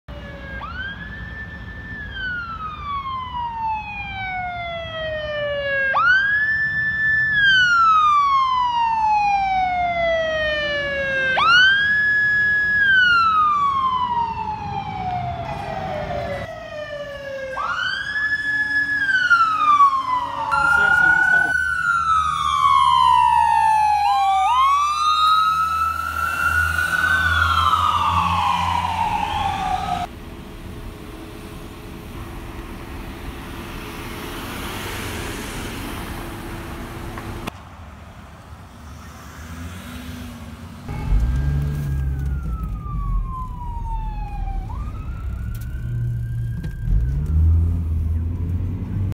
Response Compilation 1
Here are multiple short clips from the past few months
MICU 19-1 Responding
MICU 19-1 Responding
MICU 254 Responding
Deputy 99 Clearing a Fire Call
Station 250 Responding
Truck 99-1 and Ambulance 274 Responding